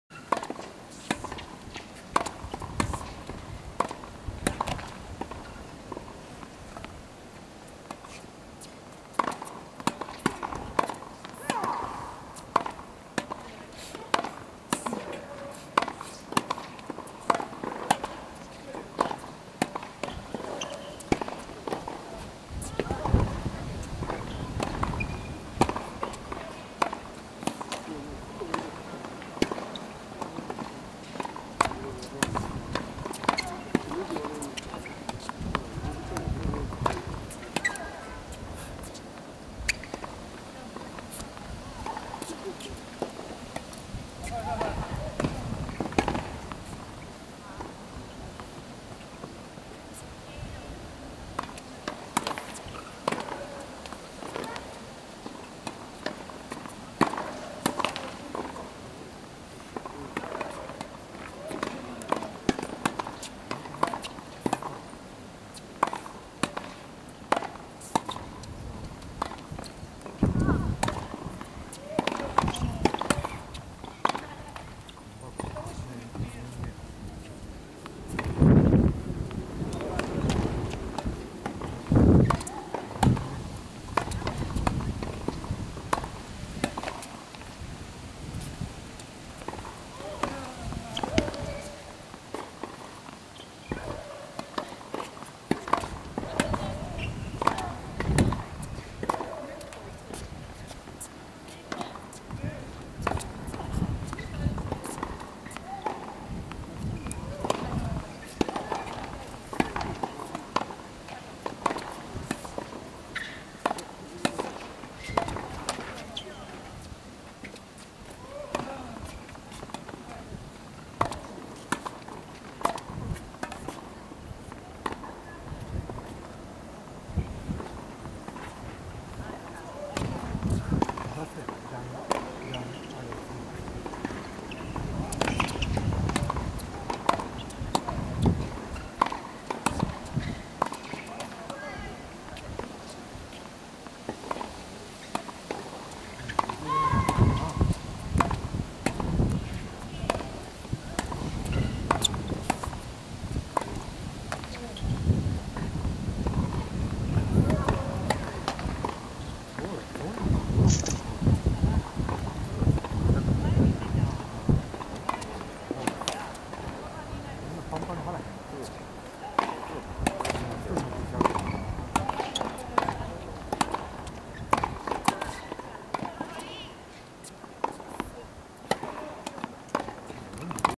クルム伊達公子ボレー練習with ザリーナ・ディアス Ｋｉｍｉｋｏ Ｄａｔｅ ｐｒａｃｔｉｃｅ ｖｏｌｌｅｙ
クルム伊達公子選手のボレー練習。練習パートナーはカザフスタンのザリーナ・ディアス。まだ調整中でネットに引っかける場面も多かったですが、さすがにボレーがうまい。ボールにラケットを合わせるのが素晴らしいタイミング。さすがに超一流。

Ｋｉｍｉｋｏ　Ｄａｔｅ　ｐｒａｃｔｉｃｅ　ｖｏｌｌｅｙ with Zarina DIYAS